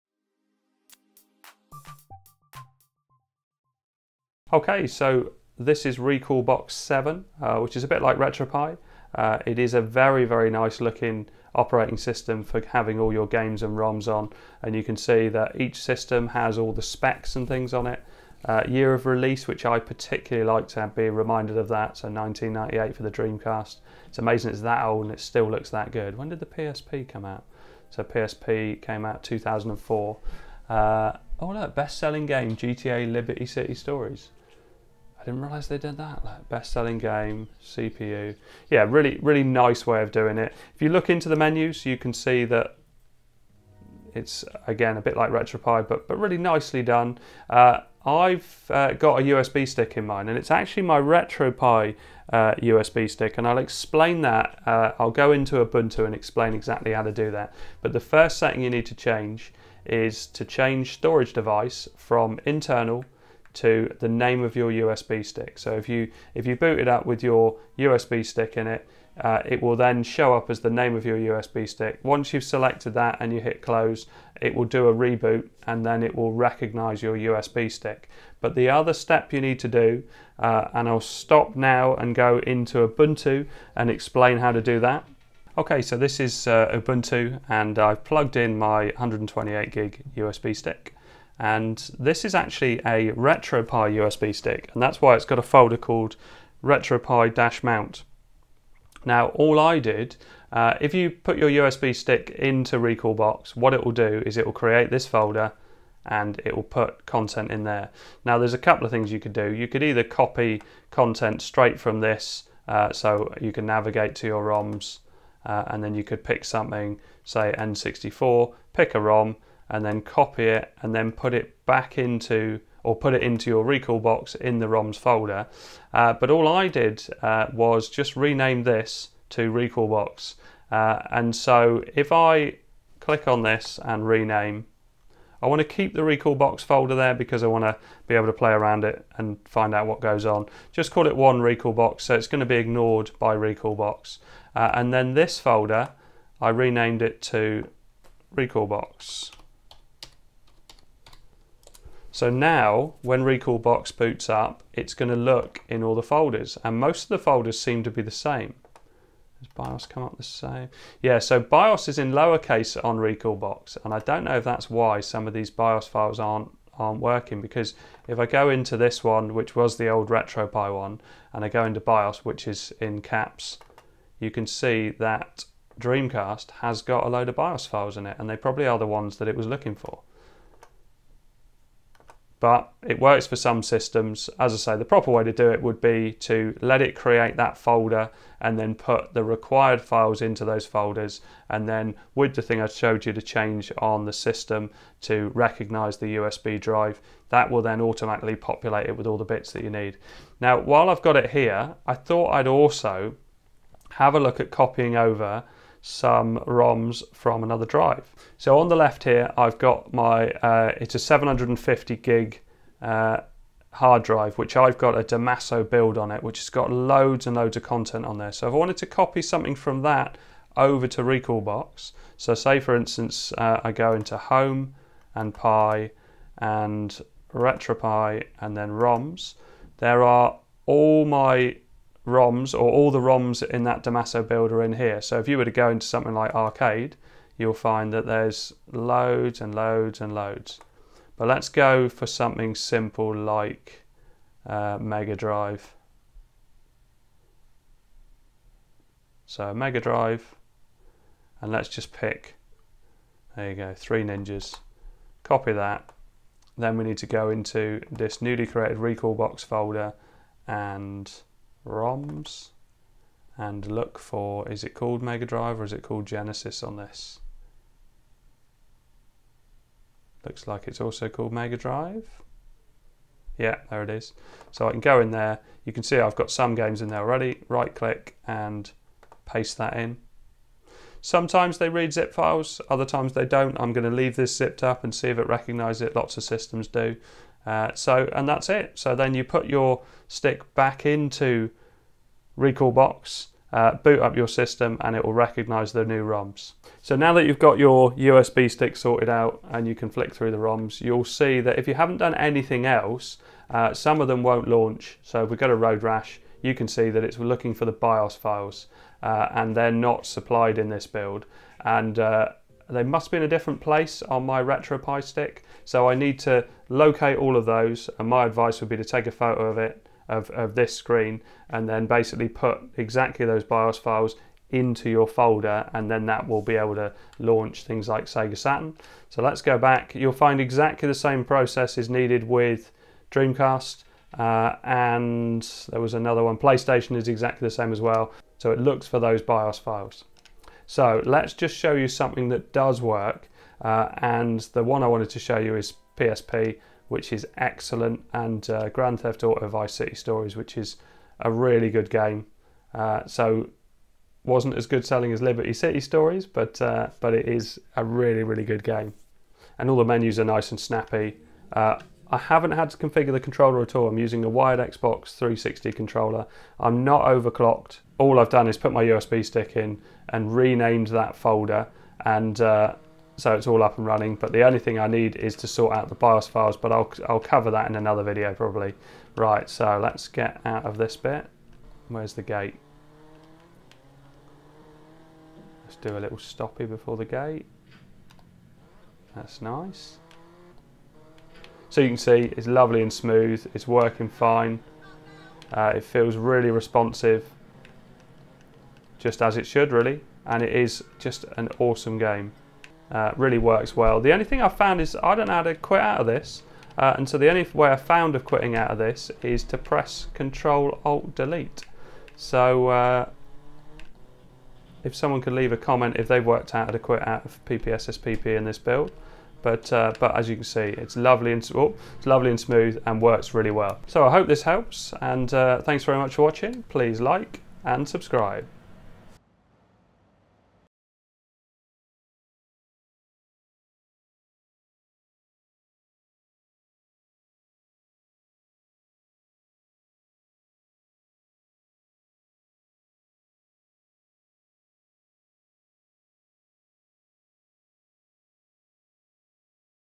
Recalbox 7 Beta Raspberry Pi 4. How to add games from USB stick. Multi Game emulator.
Recalbox 7 Raspberry Pi 4. How to add games from USB stick. Multi Game emulator.

This is a Beta version don’t judge the quality at this early stage. If like me you just want to try out new things on your Pi, go ahead.

My previous Video explaining the download.

Video soon.

All 160 plus of my Pi videos are here